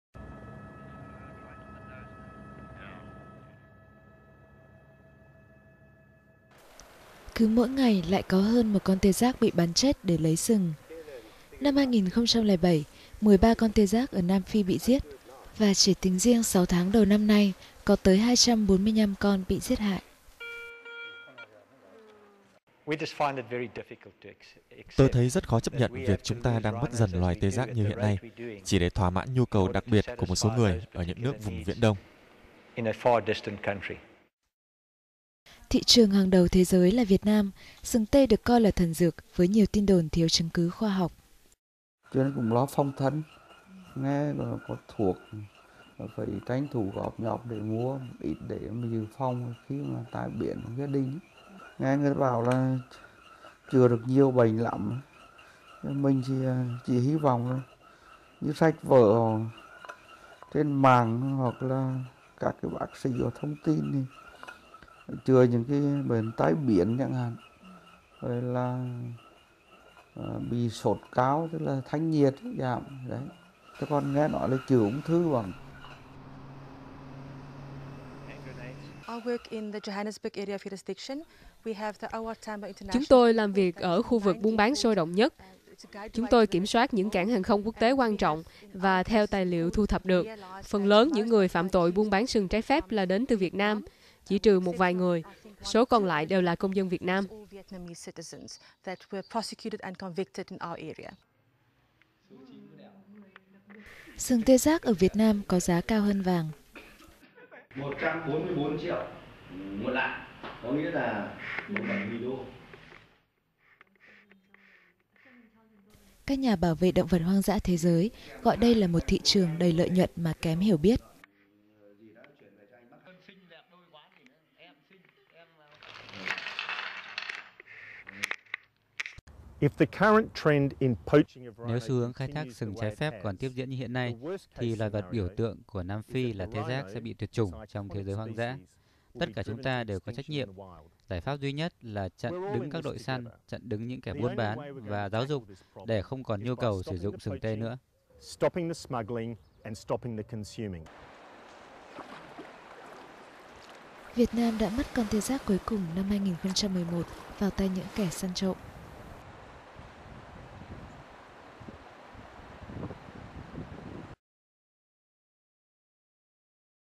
Việt Nam: Sừng tê trái phép đắt hơn vàng
Các nhà bảo vệ động vật hoang dã thế giới gọi Việt Nam là 'thị trường đầy lợi nhuận mà kém hiểu biết.'